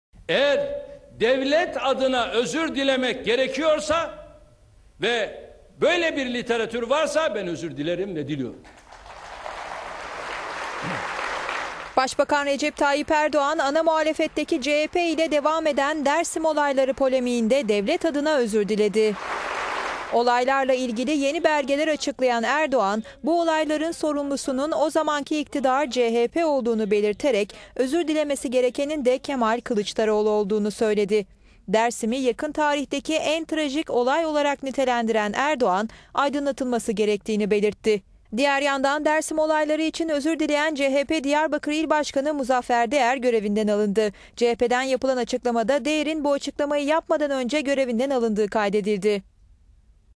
Erdoğan'dan Dersim özrü
Başbakan Recep Tayyip Erdoğan ana muhalefetteki CHP ile devam eden "Dersim olayları" polemiğinde, devlet adına özür diledi

"Eğer devlet adına özür dilenecekse, böyle bir literatür varsa ben özür dilerim, diliyorum."

Olaylarla ilgili yeni belgeler açıklayan Erdoğan, bu olayların sorumlusunun o zamanki iktidar CHP olduğunu belirterek özür dilemesi gerekenin de Kemal Kılıçdaroğlu olduğunu söyledi.

Dersim'i yakın tarihteki en trajik olay olarak nitelendiren Erdoğan, aydınlatılması gerektiğini belirtti.